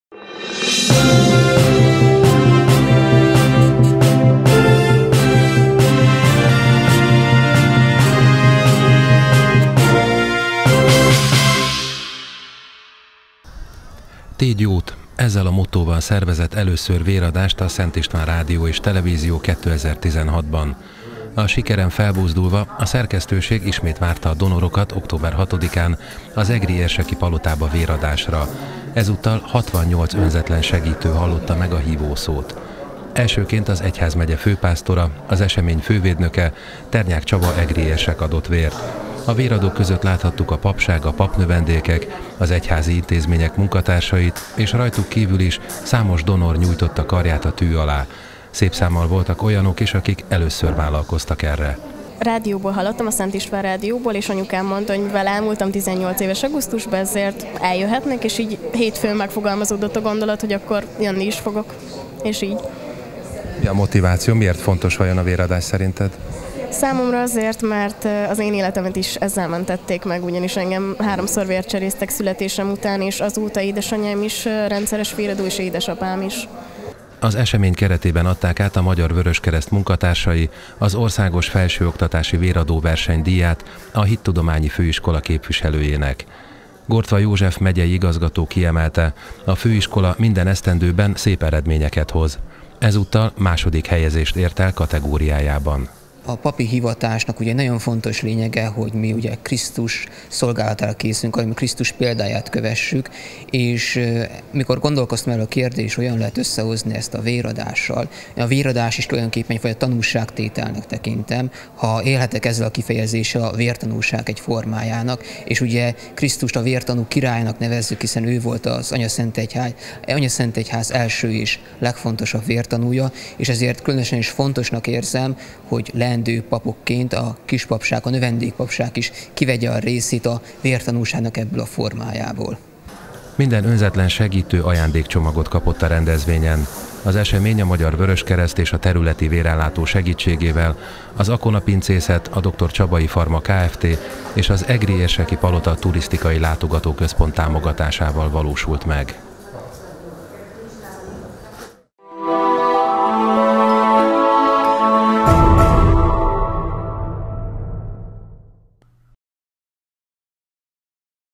Sikeres véradás
68 önzetlen segítő nyújtotta karját tű alá  a Szent István Rádió és Televízió véradásán.